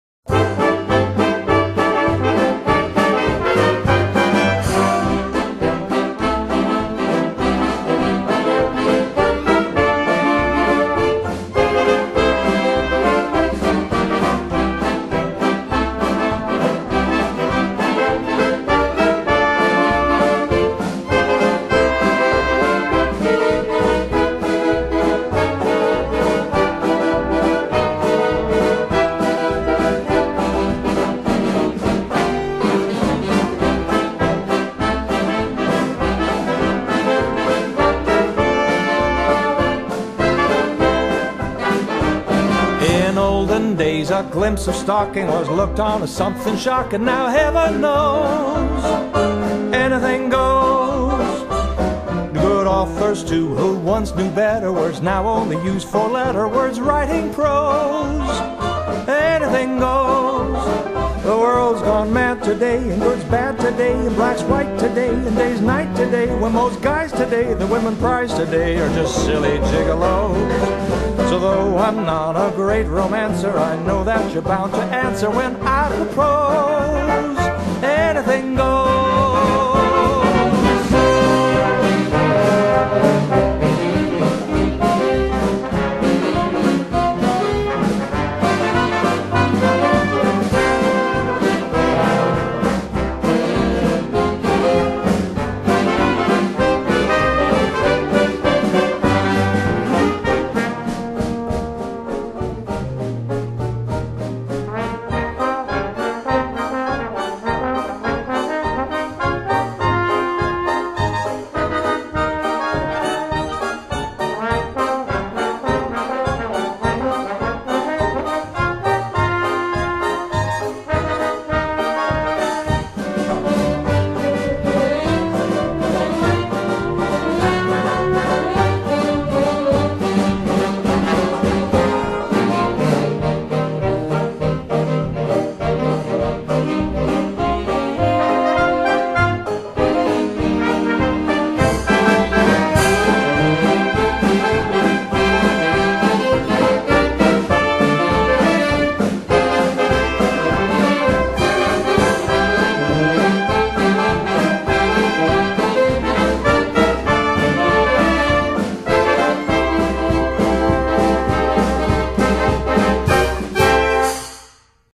Belvedere Broadcasters
A Canadian Jazz Age Dance Orchestra, The Belvedere Broadcasters are a group of fine musicians playing lively music from the 1920s and 30s. Based in Victoria, BC.